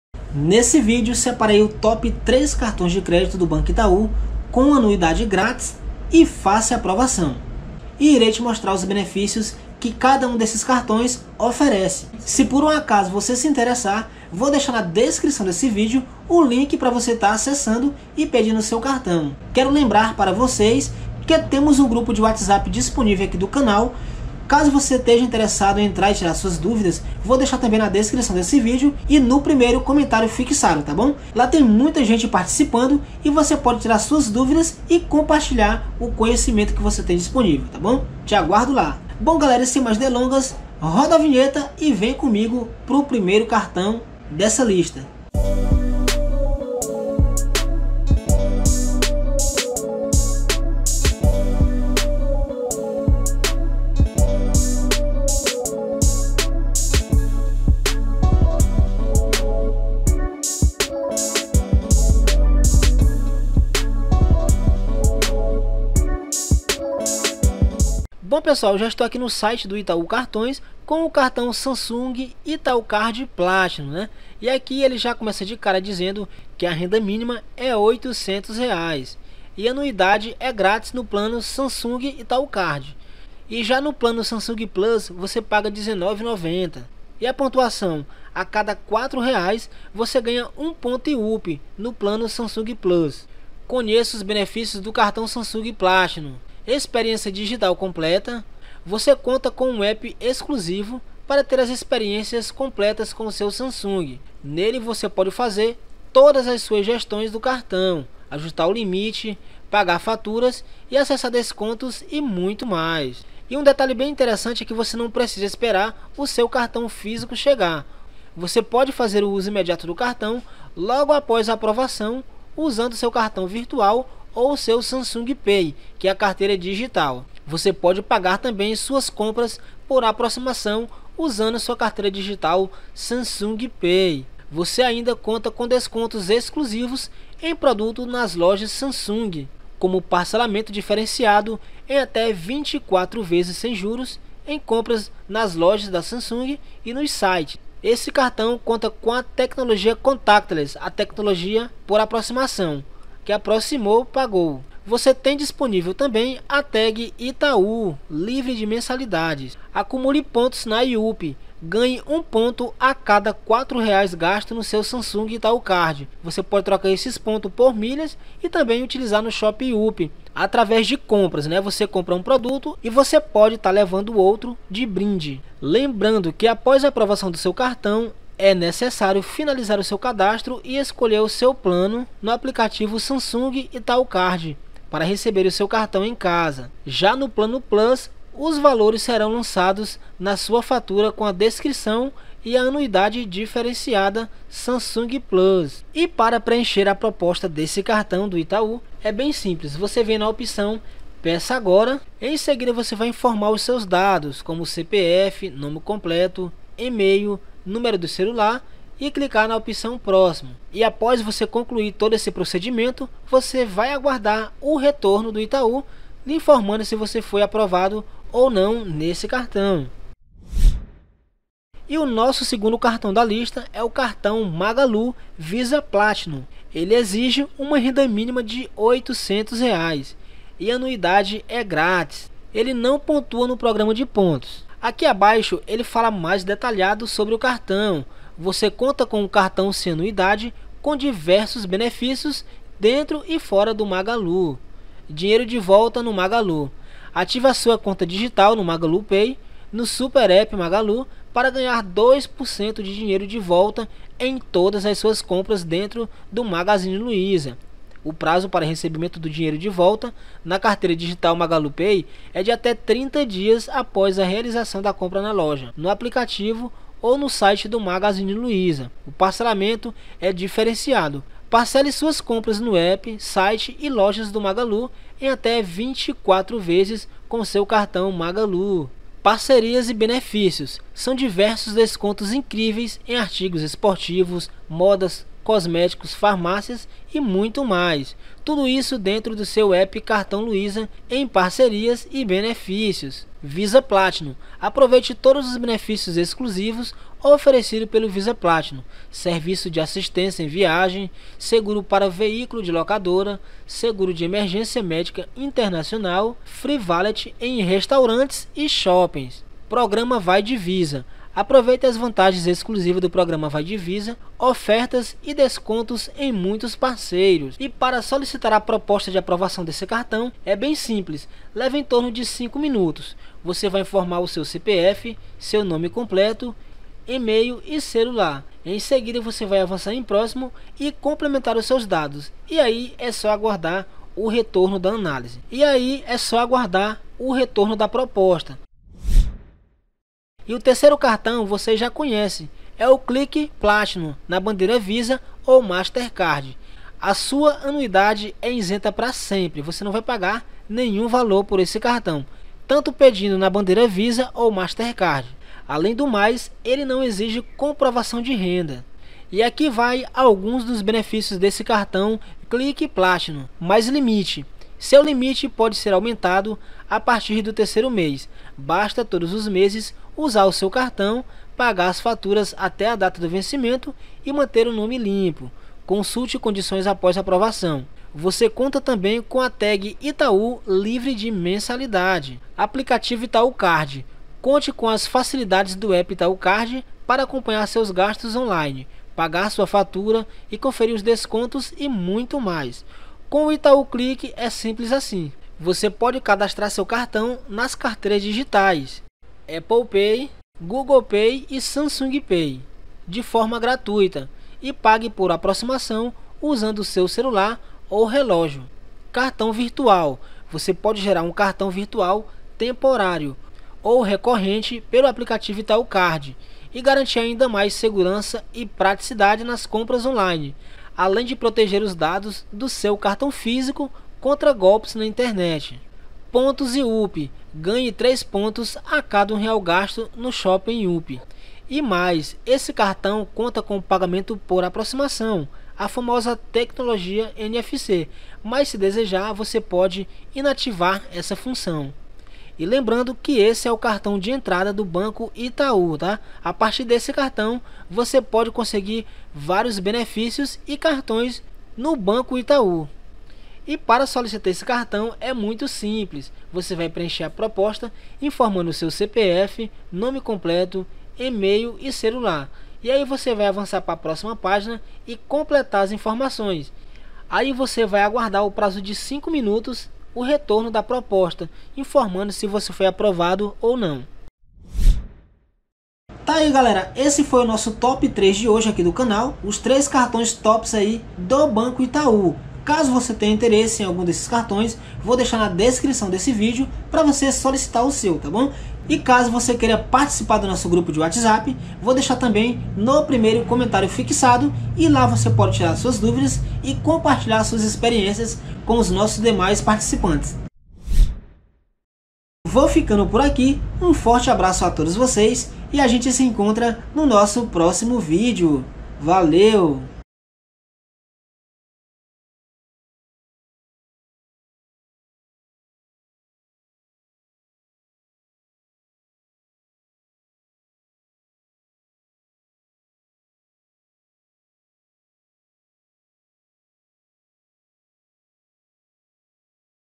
🔴INCRÍVEL! Top 3 Cartões de Crédito Itaú Com Anuidade Grátis e Fácil Aprovação em 2022, Confira
HOJE TE MOSTRO O TOP 3 CARTÕES CARTÕES DE CRÉDITO DO ITAÚ  COM ANUIDADE GRÁTIS E FÁCIL APROVAÇÃO EM 2022.